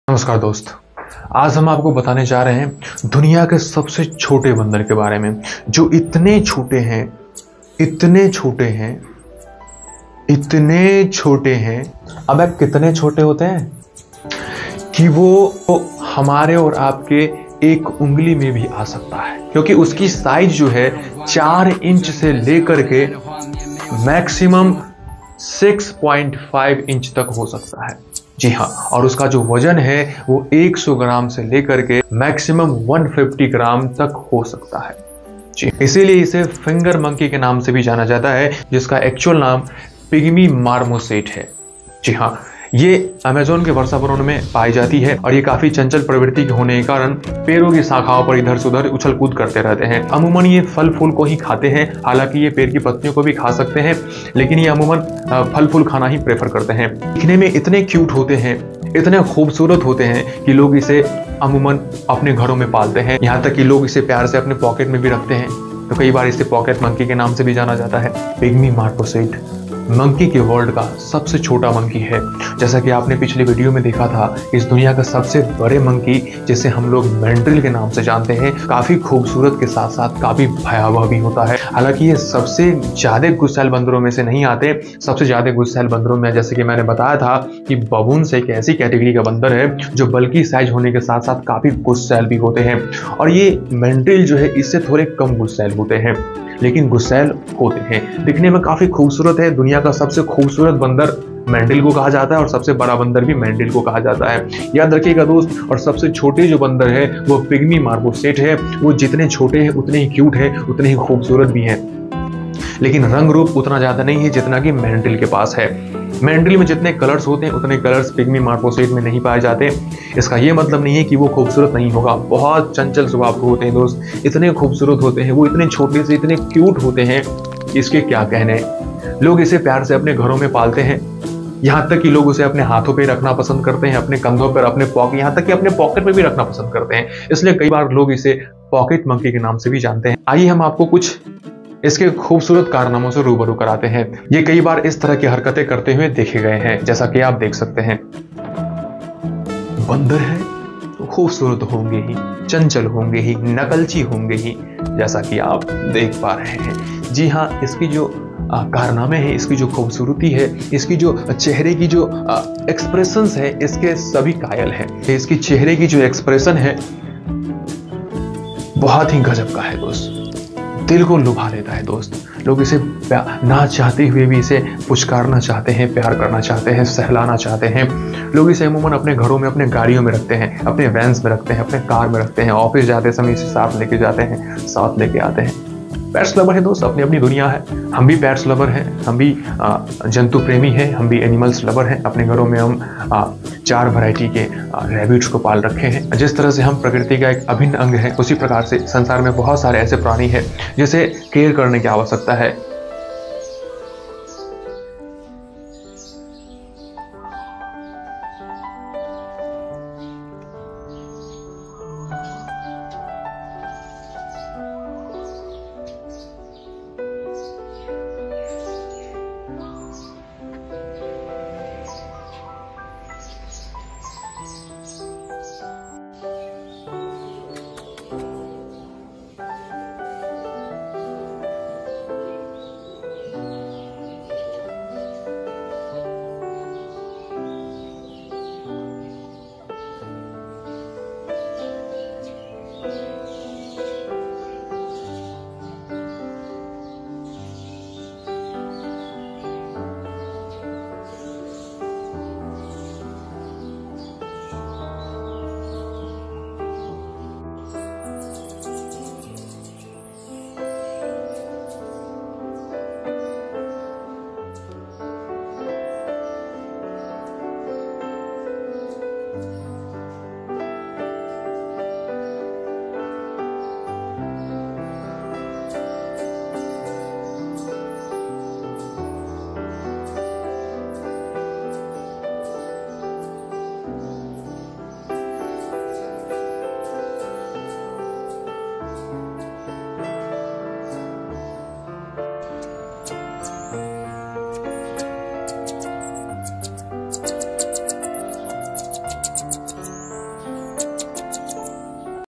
Discovered: World's Tiniest Monkey - So Adorable, You Won't Believe Your Eyes! सबसे छोटा monkey
दुनिया का सबसे छोटा monkey Smallest monkey of the world
pigmy marmoset duniya ka sabse chhota bandar hai